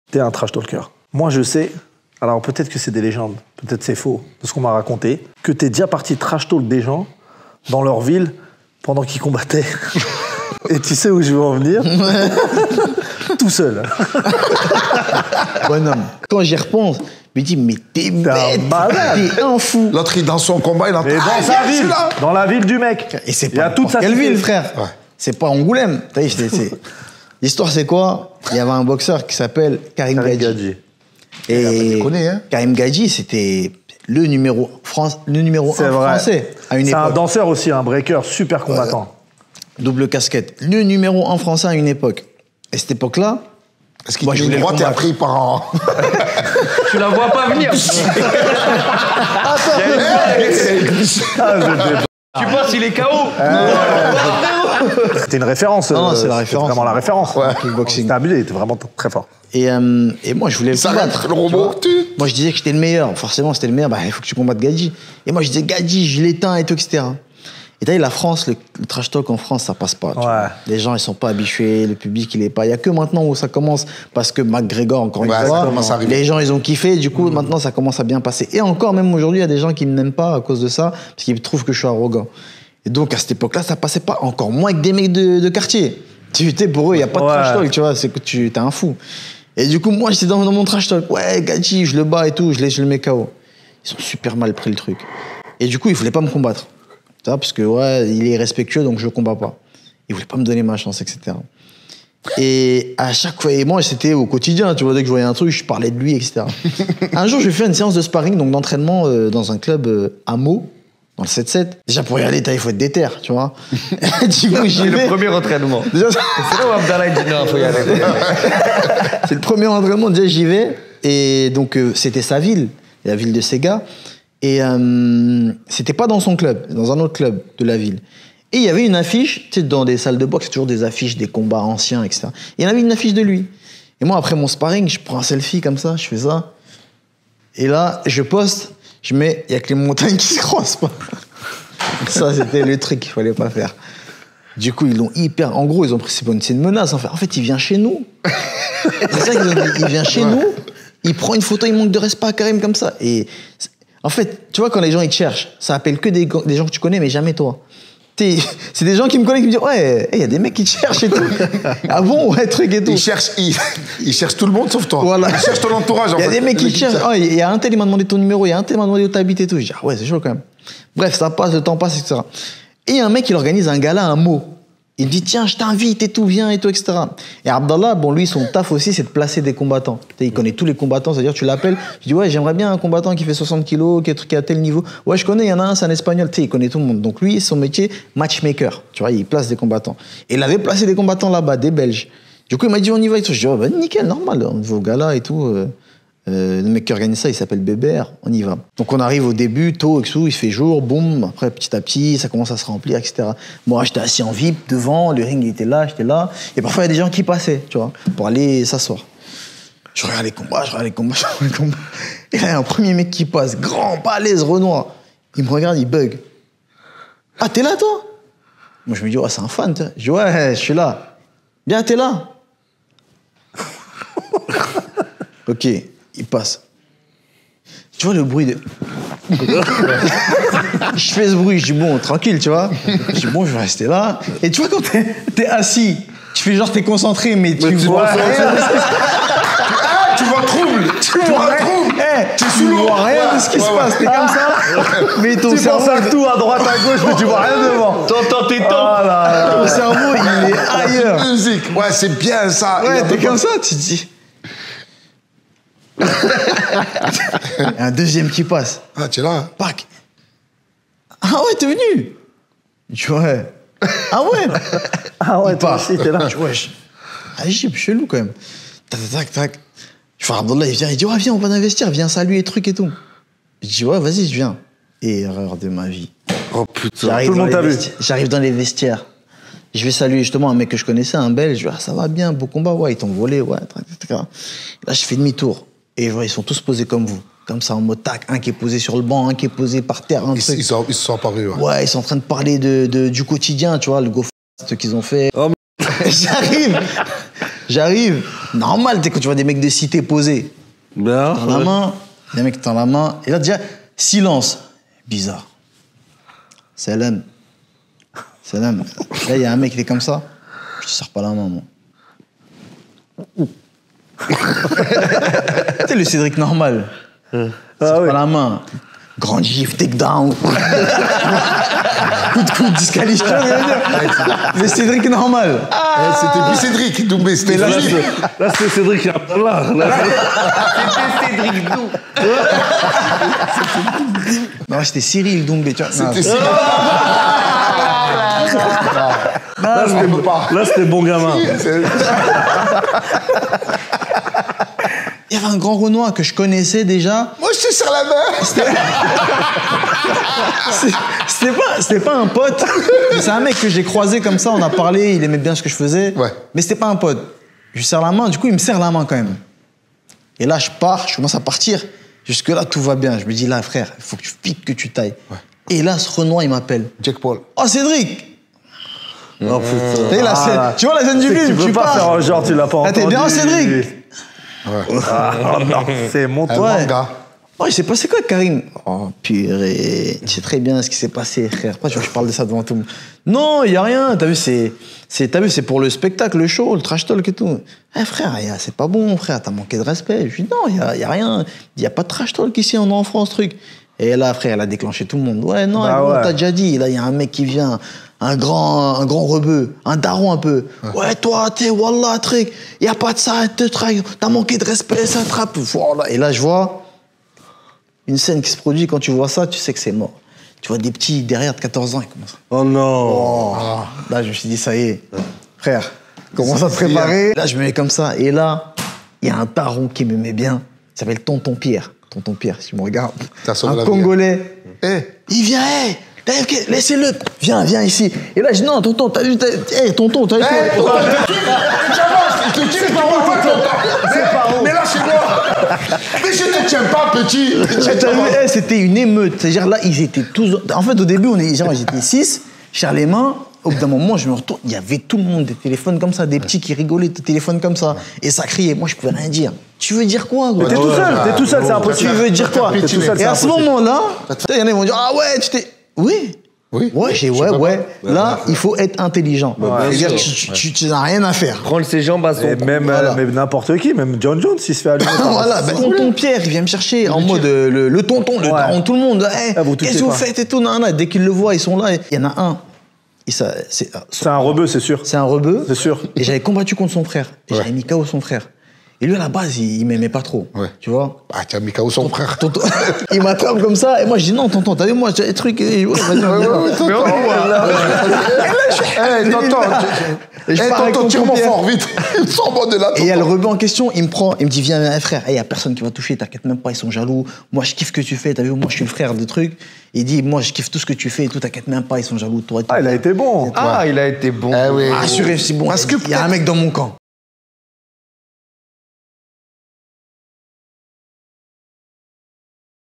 Cédric Doumbé : quand le trashtalk a failli se transformer en combat !
Avant de remporter son combat contre Jordan Zebo, Cédric Doumbe, le kickboxeur français a fait le buzz avec son trashtalk lors de son combat contre Karim Ghajji. Dans cette interview exclusive, il nous raconte les mésaventures auxquelles il a fait face à cause de ses propos provocateurs. Découvrez l'envers du décor de l'univers de la boxe avec Cédric Doumbe, un sportif talentueux au franc-parler assumé.